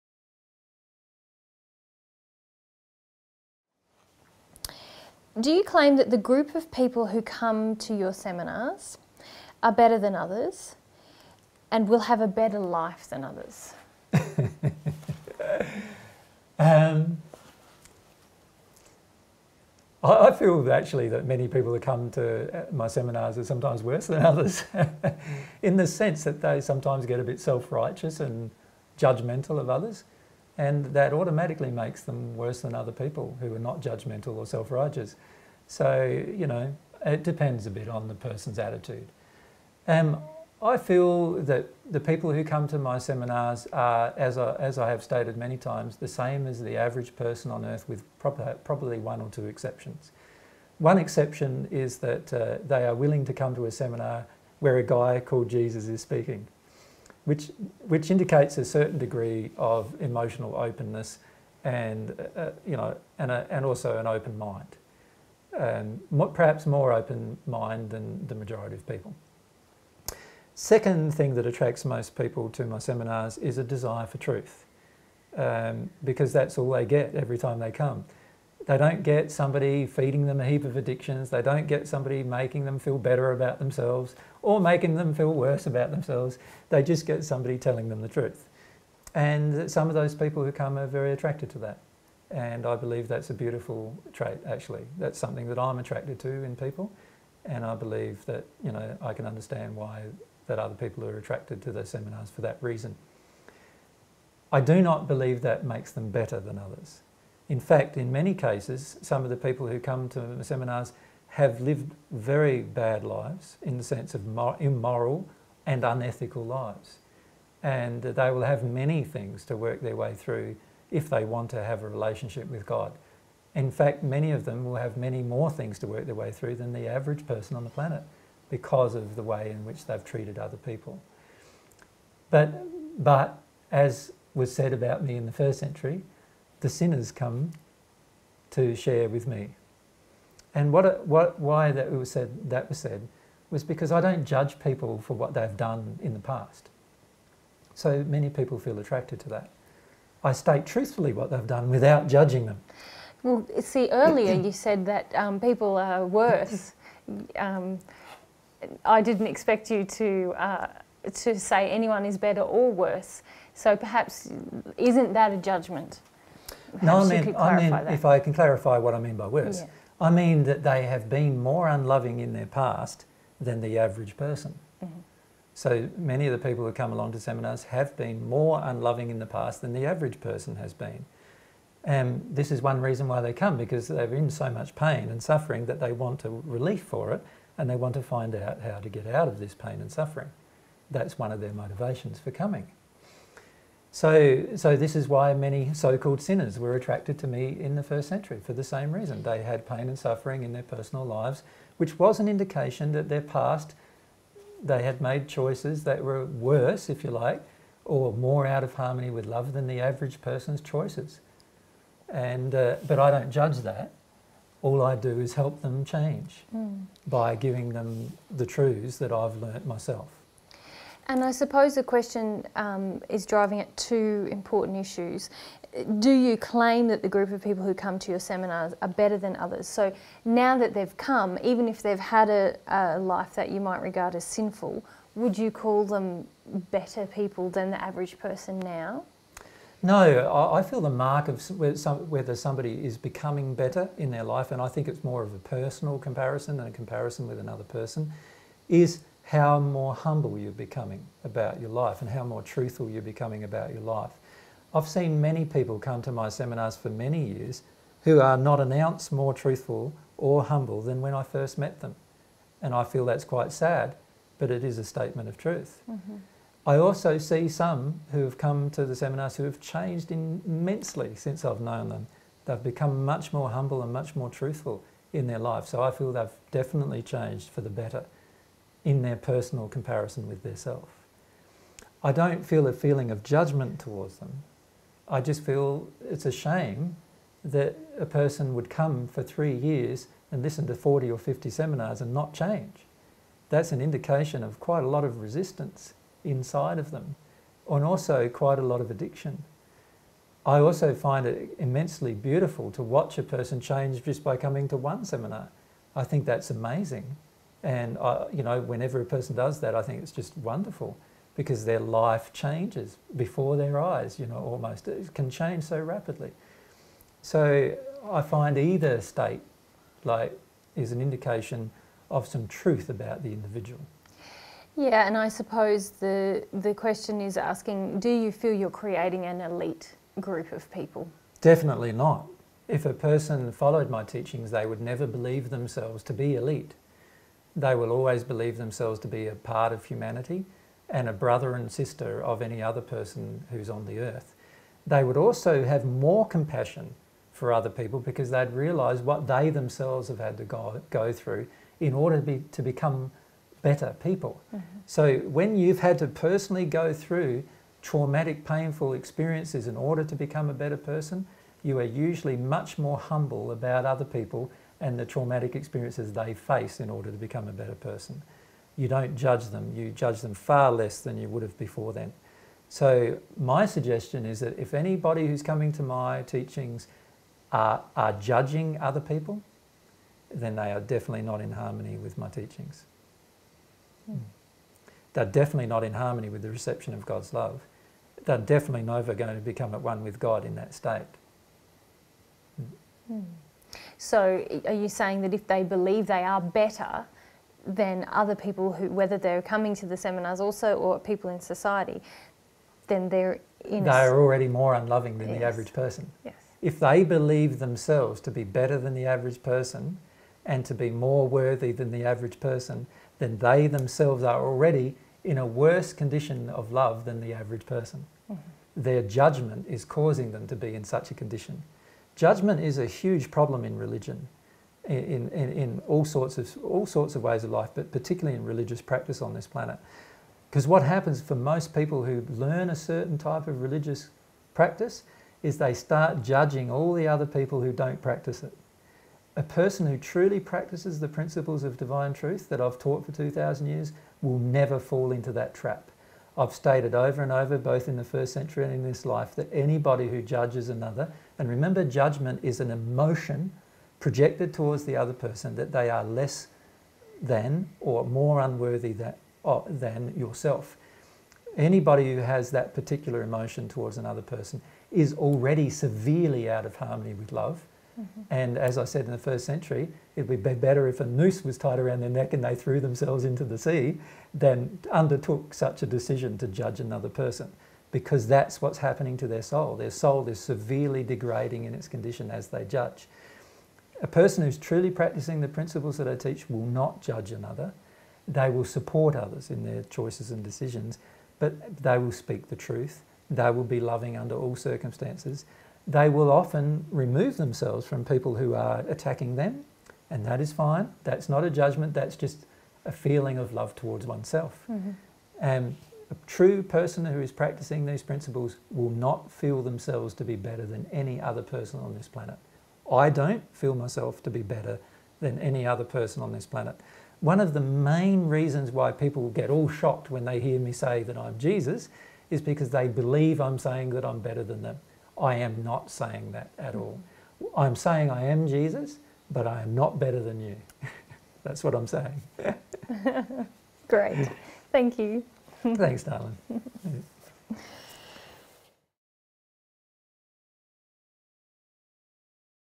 Do you claim that people who come to seminars are better than others?
31. Do you claim that the group of people who come to your seminars are better than others, and will have a better life than others?

20121230 Cults S02Q31

Recorded on 30th December 2012 in Wilkesdale QLD, Australia